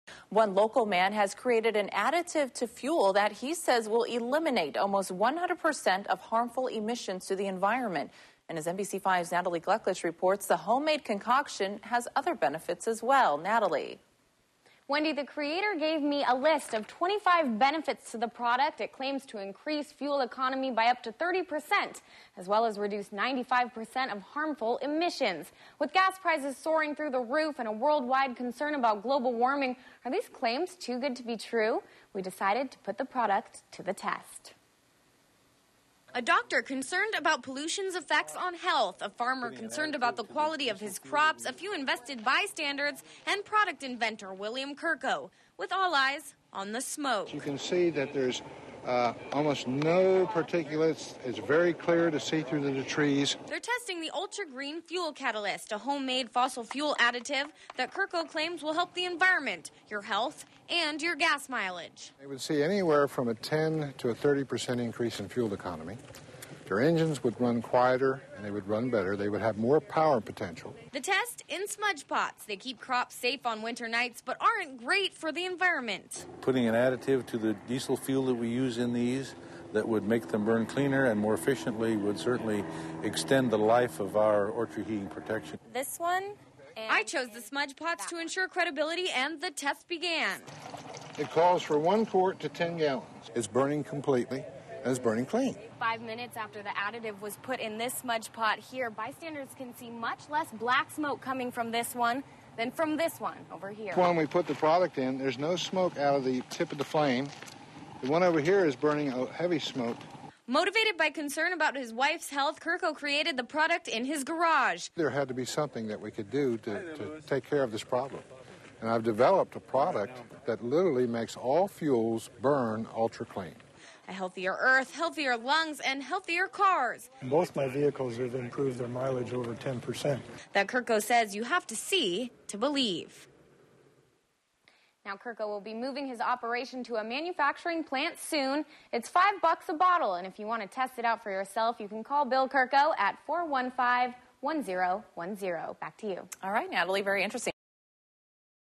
Oil companies are gonna cry now!
This stuff works in all fossil fuels and makes them burn clean with more power, and saves up to 30% in fuel! Check it out!  Oil companies are gonna cry!